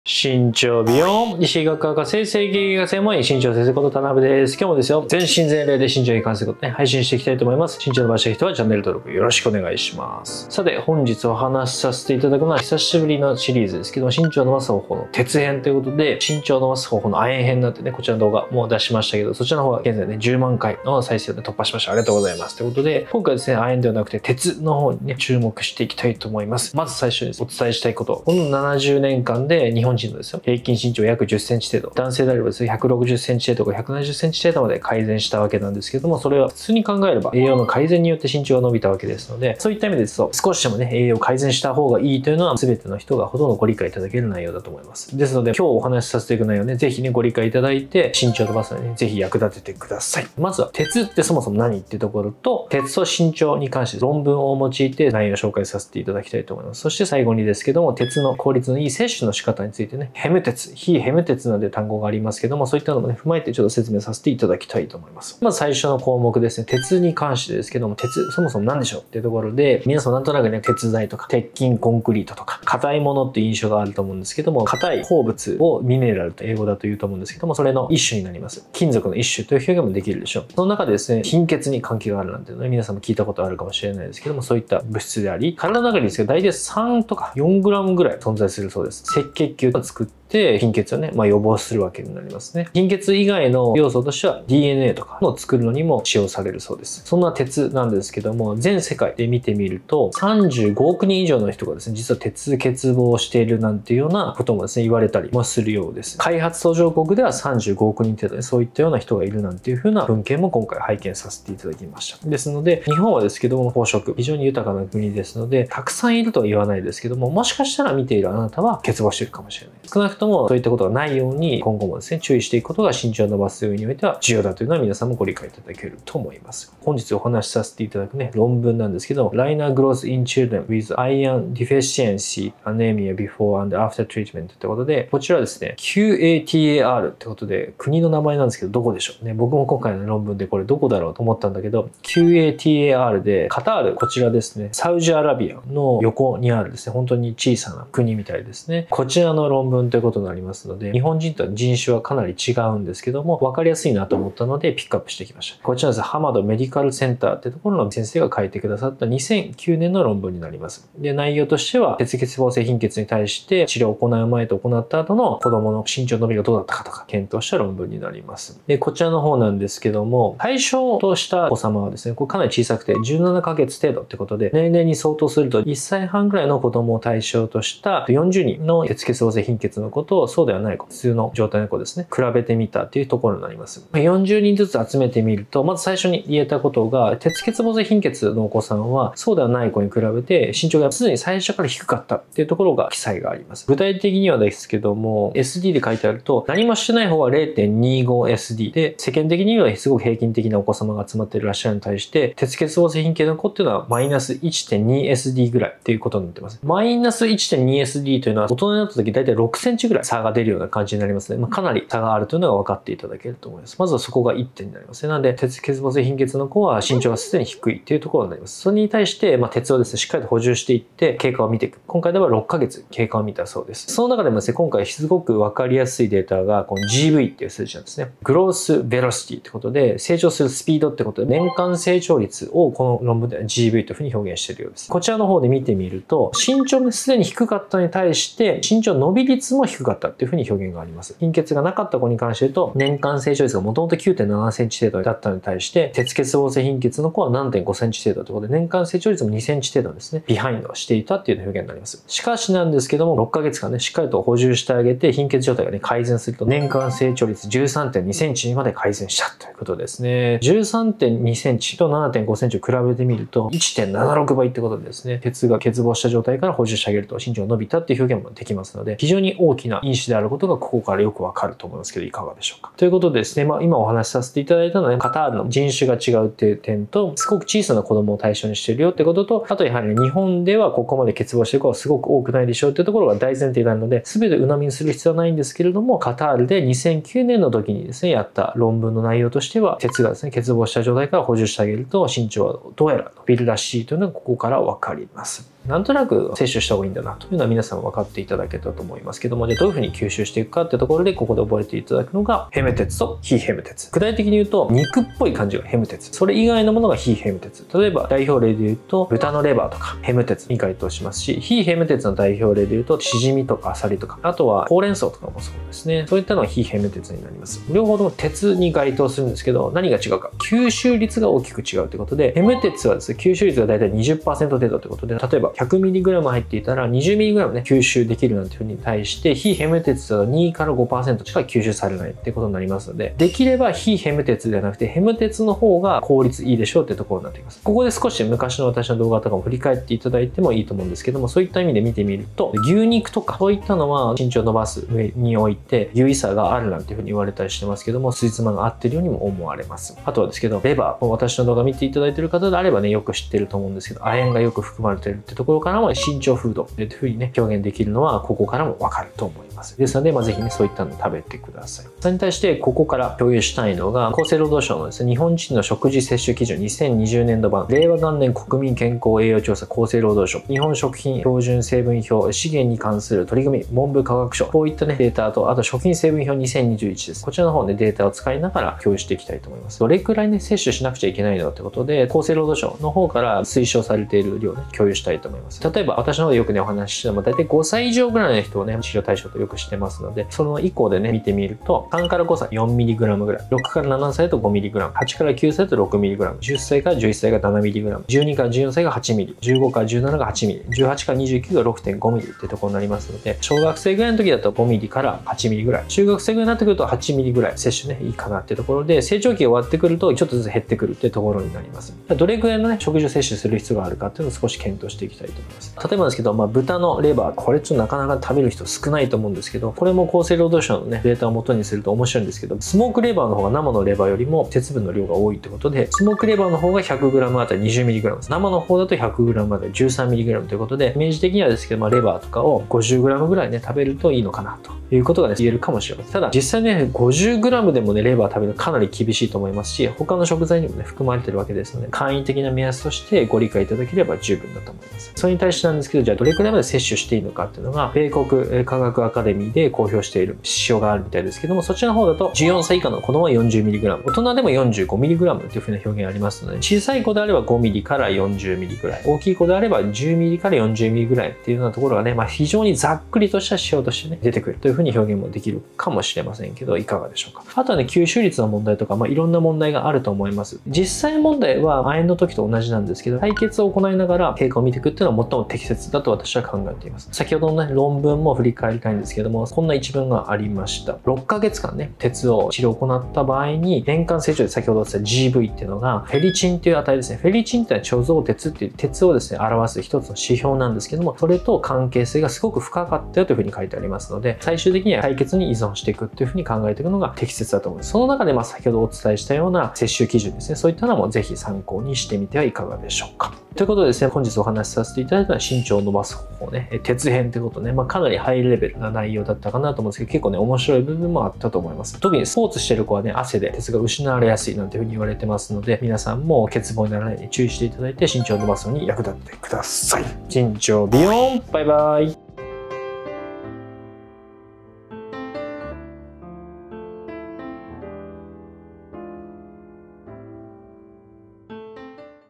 【鉄】で身長が伸びるのは本当か？副作用に注意！？【身長先生】
【亜鉛】で身長が伸びるのは本当か？副作用に注意！？【身長先生】

■目次
00:00 オープニング
00:10 動画の概要
01:05 鉄とは？
04:55 どう吸収するのか
06:01 摂取量について
08:23 エンディング